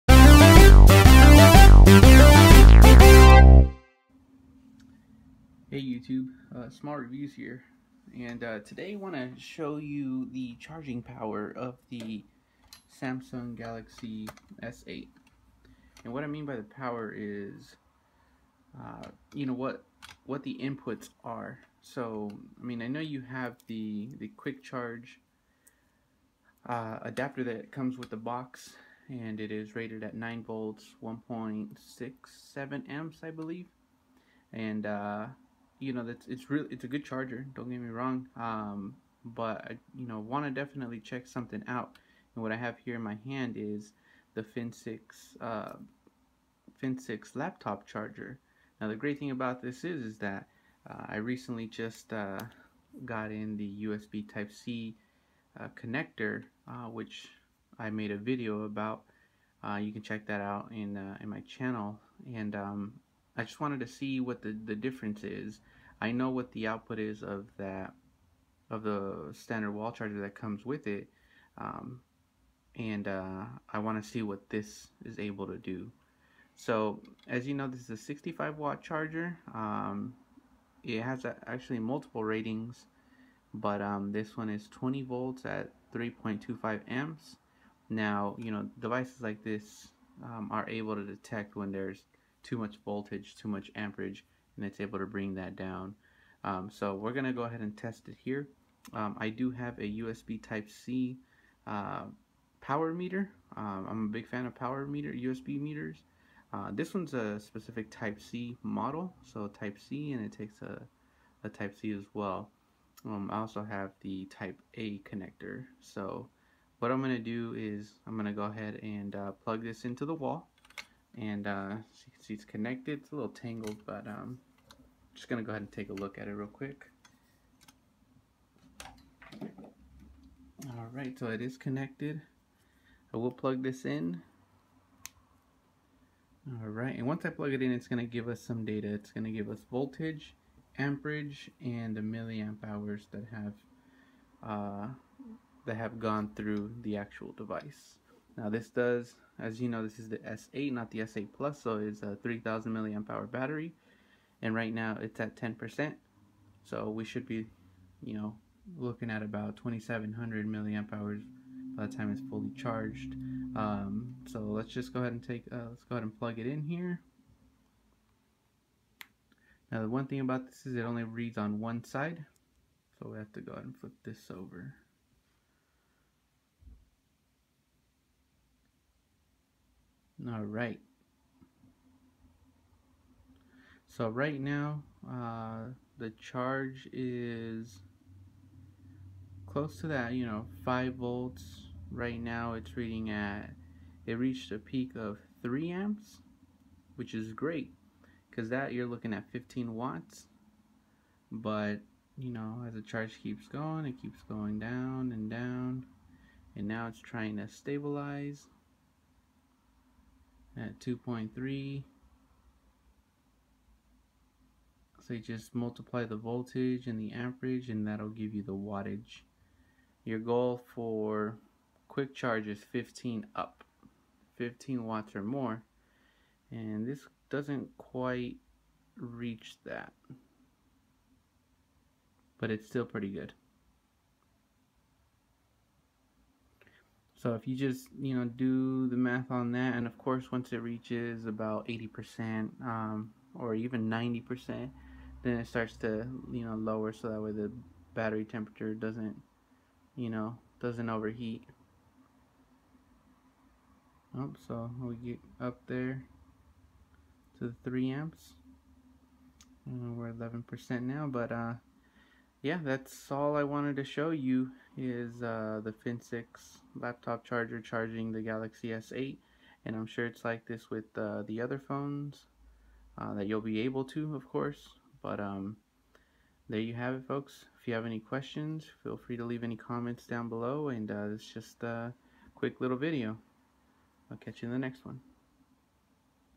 Galaxy S8 connected to 65W charger
I am attempting to charge a Galaxy S8 with a 65W laptop charger (Dart charger) to see if anything would happen